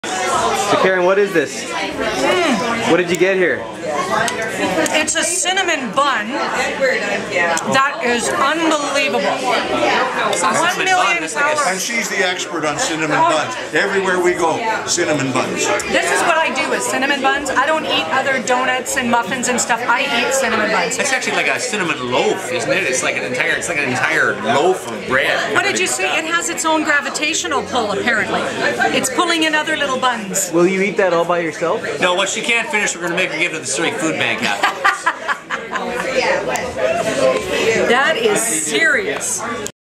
Karen Daniels - Cinnamon Bun!
Karen Daniels eating a Cinnamon Bun during the Hometown Handshake Tour at the Wired Monk in Surrey.